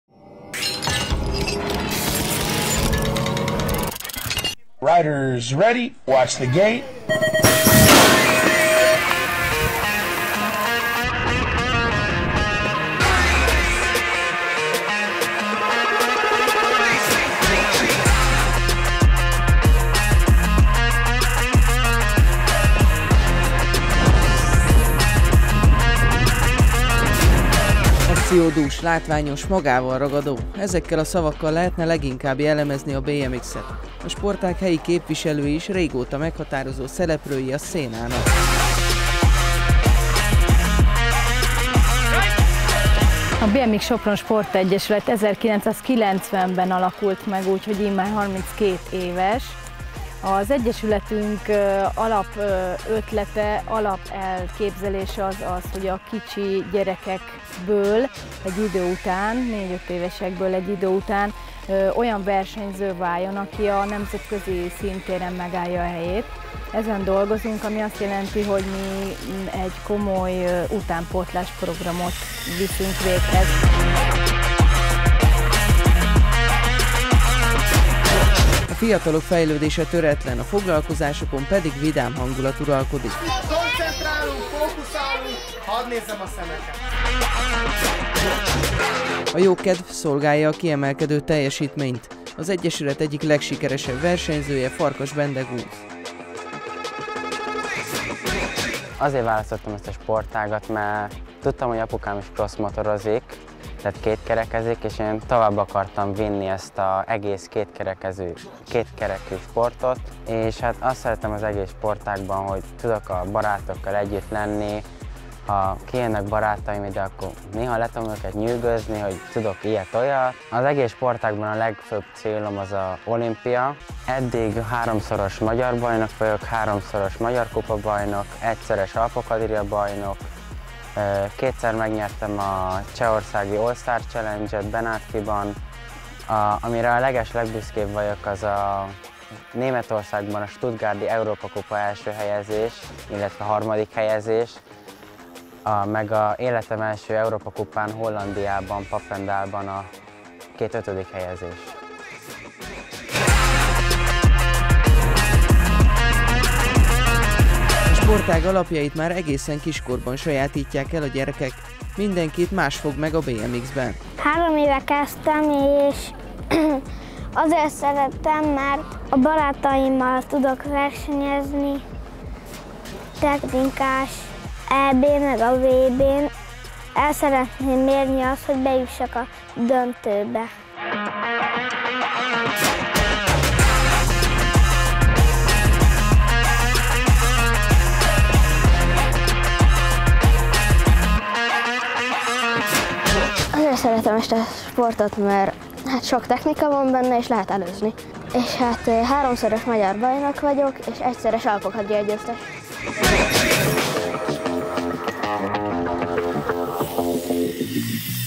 Egyesületi bemutató: BMX Se Sopron
Folytatjuk egyesületi bemutatóinkat. Ezúttal a BMX Se Sopron fiataljaihoz látogattunk el.